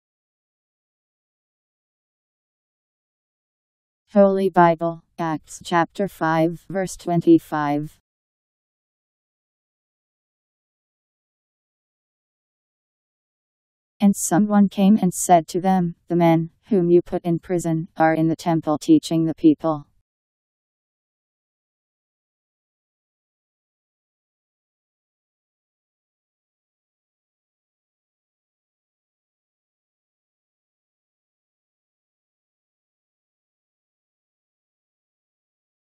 Holy Bible Acts 5:25
Video shows Holy Bible Acts Chapter 5 Verse 25: And someone came and said to them, The men, whom you put in prison, are in the Temple teaching the people. Powered by MaryTTS. Text taken from Bible in Basic Edition. Bible Acts 5:25 Bible Learning, what Bible verse says.